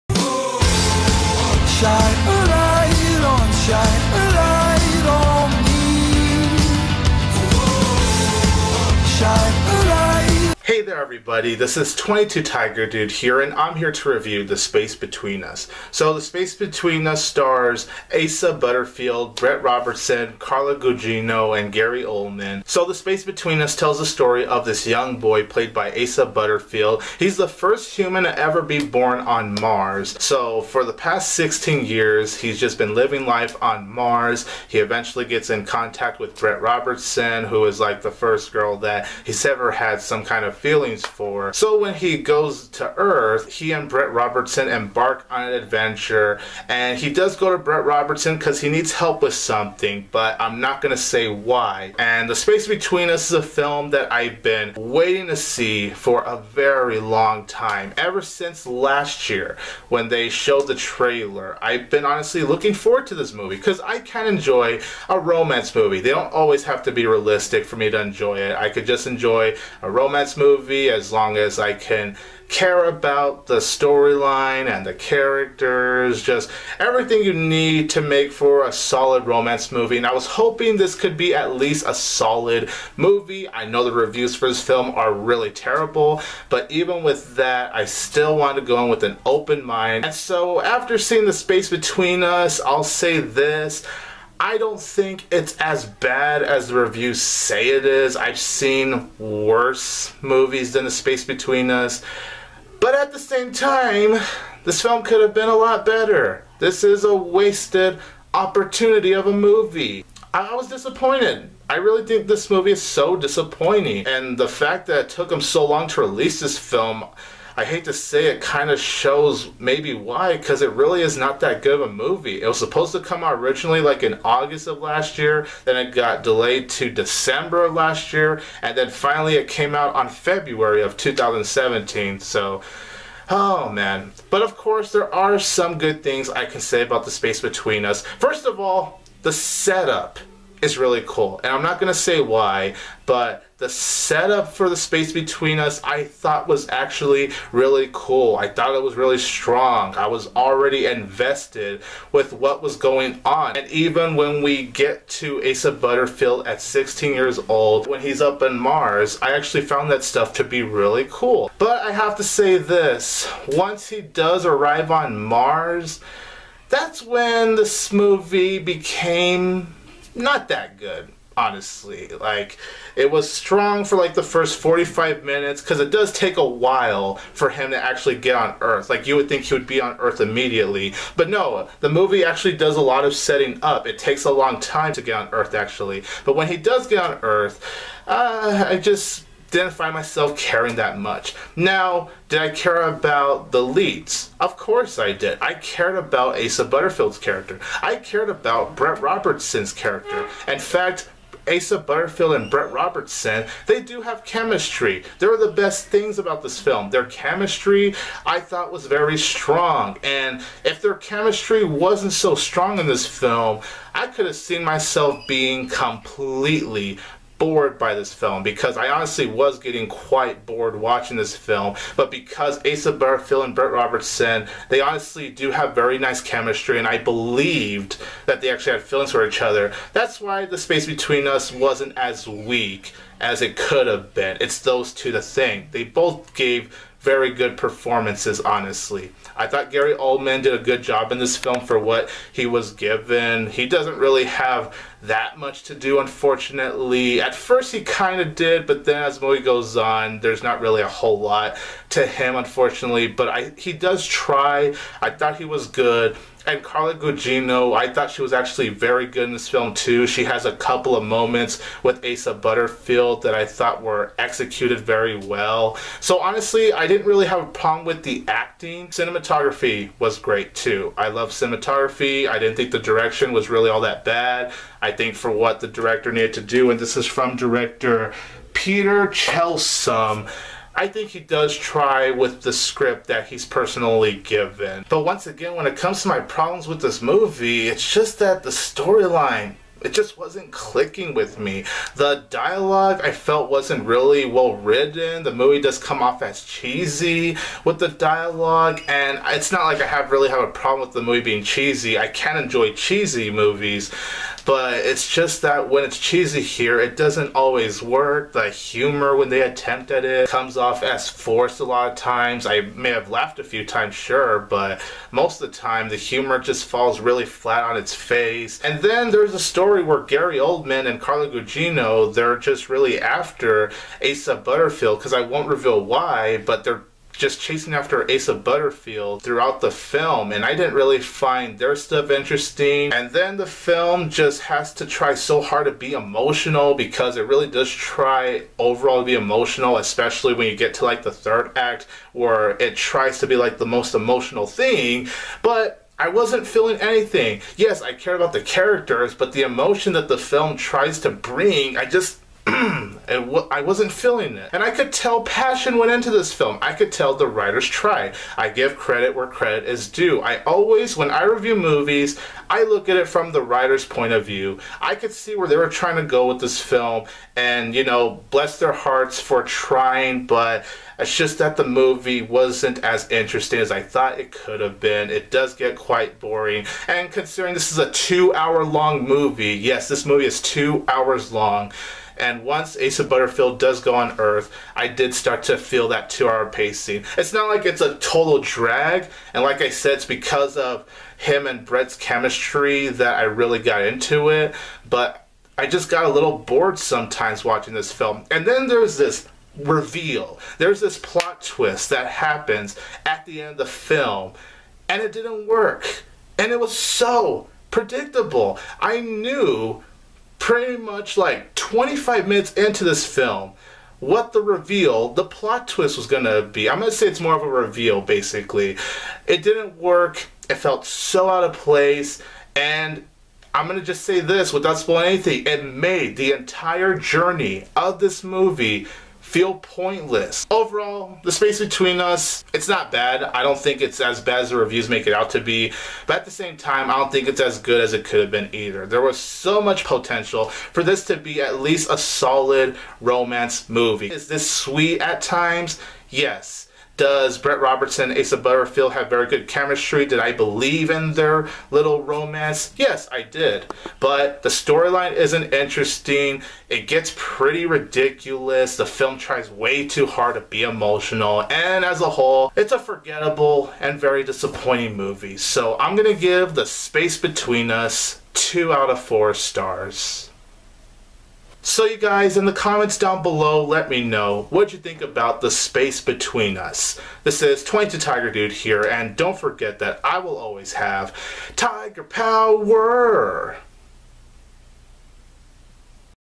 The Space Between Us movie review
22TigerDude reviews the romance drama adventure film starring Asa Butterfield, Britt Robertson, Carla Gugino, and Gary Oldman. What are his thoughts on this film? find out here.